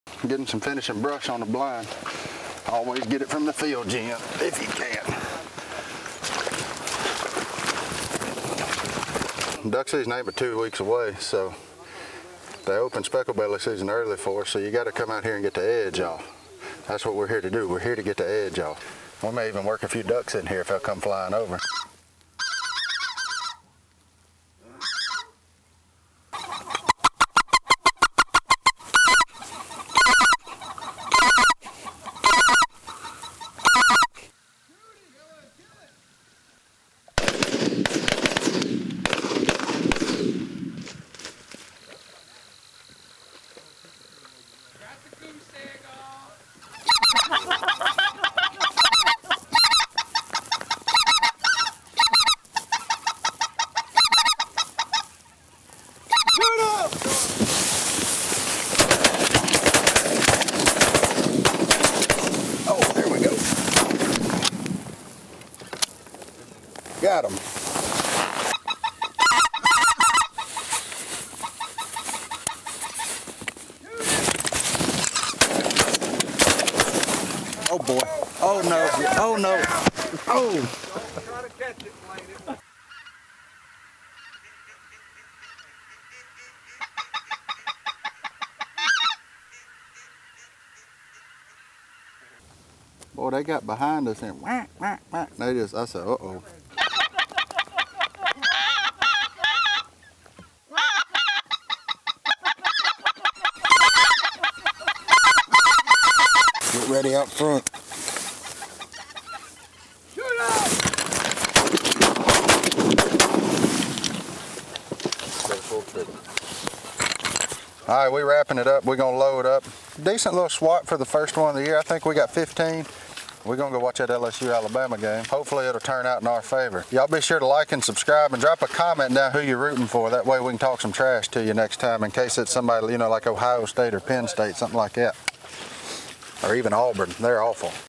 Goose Season Opening Day in Louisiana with Justin Martin
We're warming up for duck season here in Louisiana with a few specklebellies.

►Are you ready for duck season?